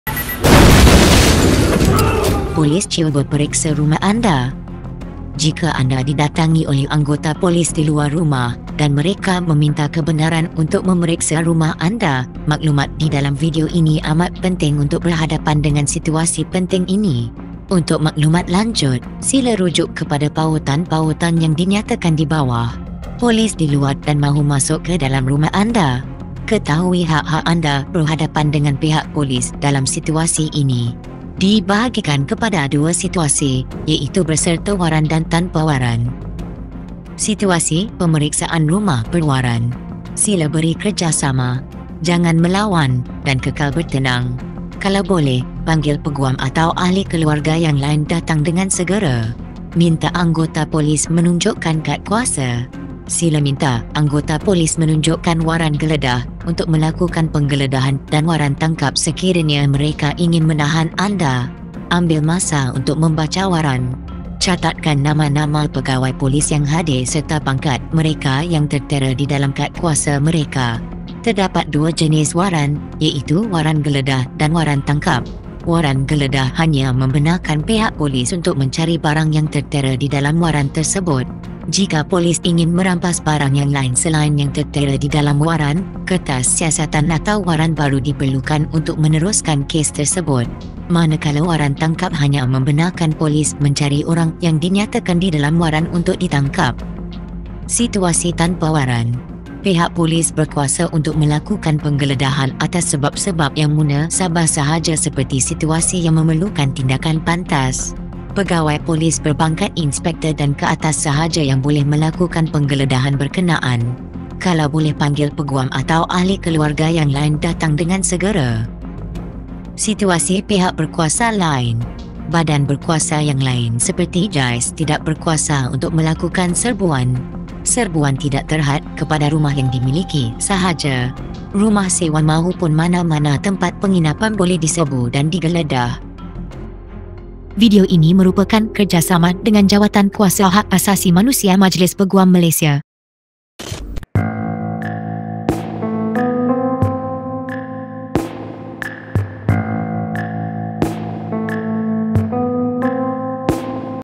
Rumah Diserbu PDRM? Video Ni Boleh Bantu Anda!
PDRM datang ketuk pintu Rumah? Apa patut anda buat? Video ni boleh selamatkan anda!

Video ini merupakan kerjasama dengan Jawatankuasa Hak Asasi Manusia Majlis Peguam Malaysia